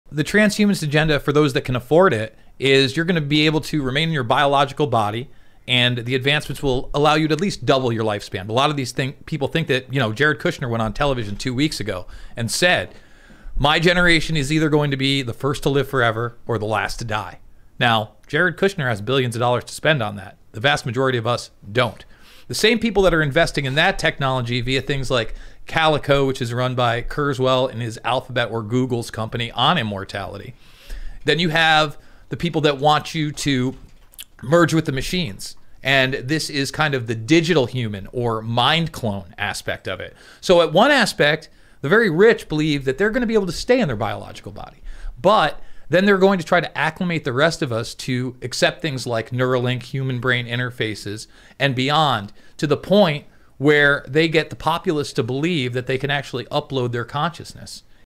Timcast IRL - What Is The Transhumanist’s Agenda?
Hosts:
Tim @Timcast (everywhere)
Ian @IanCrossland (everywhere)
Luke @WeAreChange (YouTube)
@LukeWeAreChange (Twitter)
Lydia @sourpatchlyds (Twitter), sourpatchlyds.me